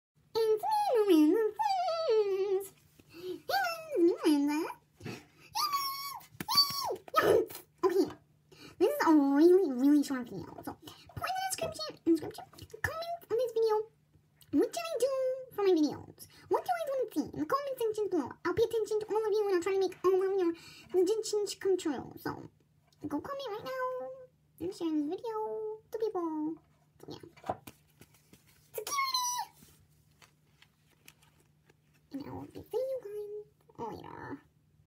WHAT SHOULD I DO
Hey guys! In this video Miranda comes back for a favor. She ask us to put comments about what videos me and her should make. Go comment!
                   -Melodyxoxo